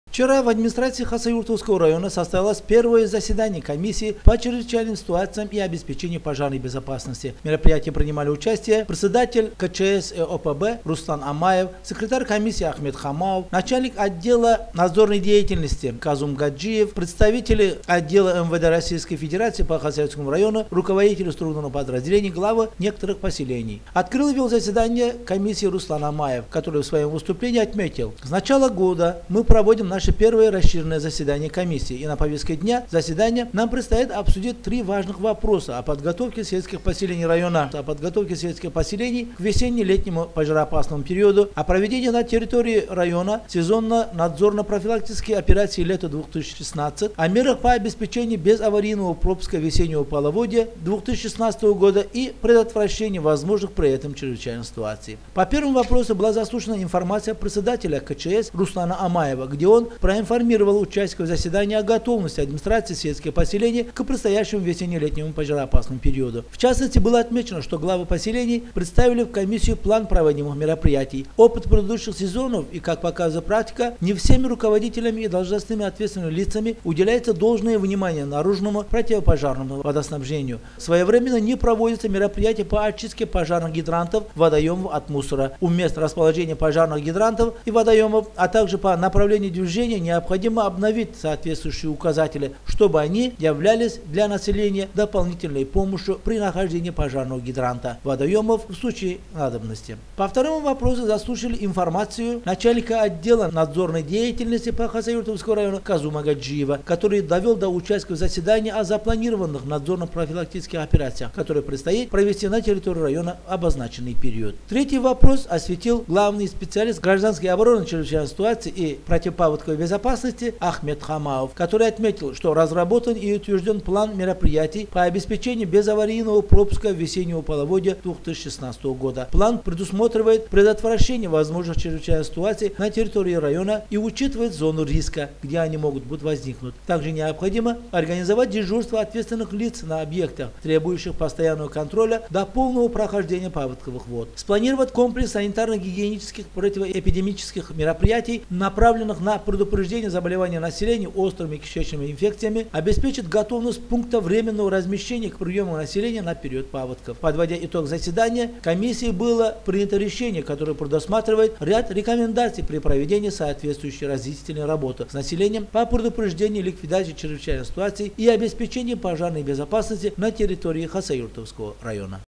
В Хасавюртовском районе состоялось первое заседание Комиссии по предупреждению и ликвидации ЧС
В Хасавюртовском районе состоялось первое заседание Комиссии по предупреждению и ликвидации ЧС и обеспечению пожарной безопасности.